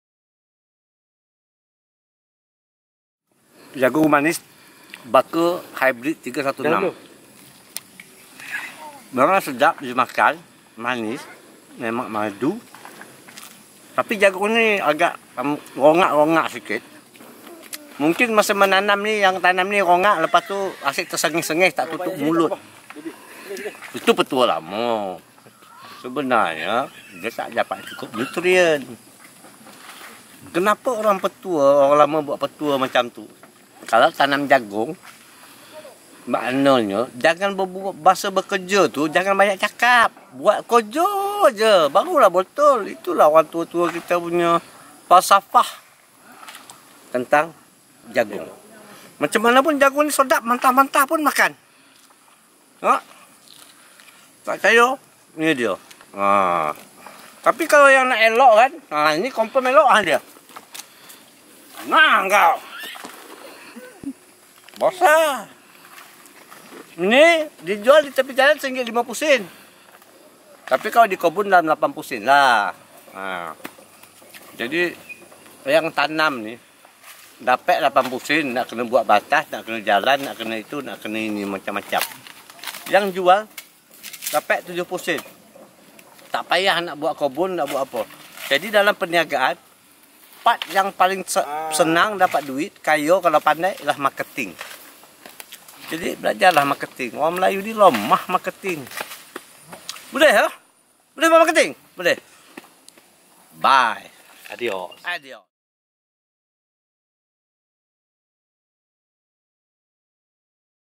Apa kes ni.. Tanam Jagung Rongak??.... tapi manis.. sedap..
Sembang pasal jagung dengan Pak Faisal KS
Cerita pasal tanam jagung dan jual jagung.
Jagung sedap dan Jagung manis